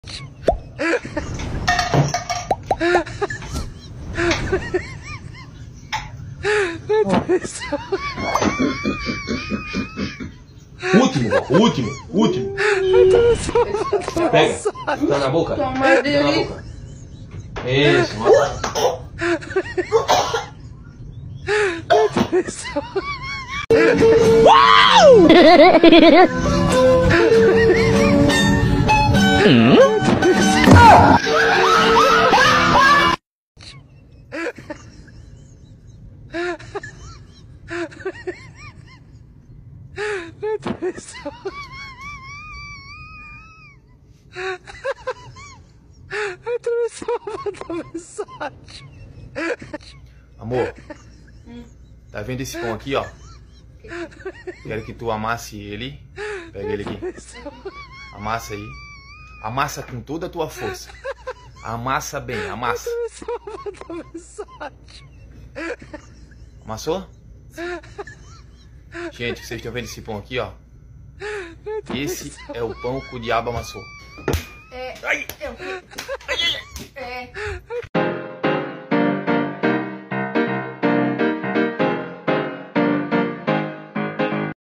COMÉDIA DE CASAL - VÍDEOS ENGRAÇADOS DE CASAL *Tente Não RIR*
No vídeo de hoje muita comédia em casal, um pequeno resumo dos nossos vídeos mais engraçados em casal, um pouco de como levamos a vida com muita alegria e humor, a afinal um dia sem rir é um dia desperdiçado!
Assista também nosso último vídeo, abaixo segue o link do nosso último vídeo: